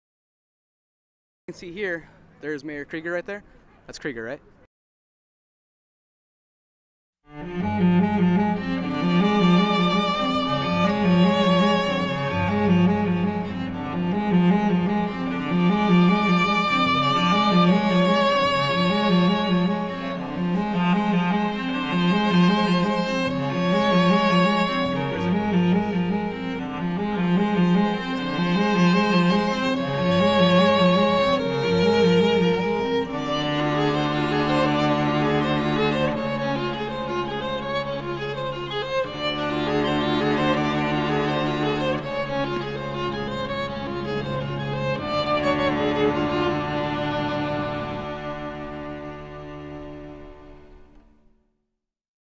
Yuma Prison short HD
Shot at Yuma Territorial Prison on March 10.

Mostly done to test HD compression on You(tube)